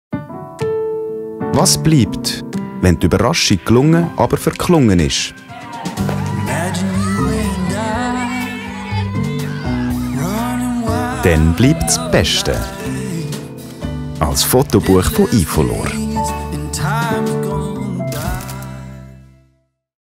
ifolor Inspiration | Geburtstagsparty vorbei? Dann bleibt das Beste.
Was bleibt, wenn die Überraschung zum Geburtstag gelungen, aber verklungen ist?​ Dann bleibt das Beste.​ Als Fotobuch von ifolor.

ifolor - Fotos fürs Leben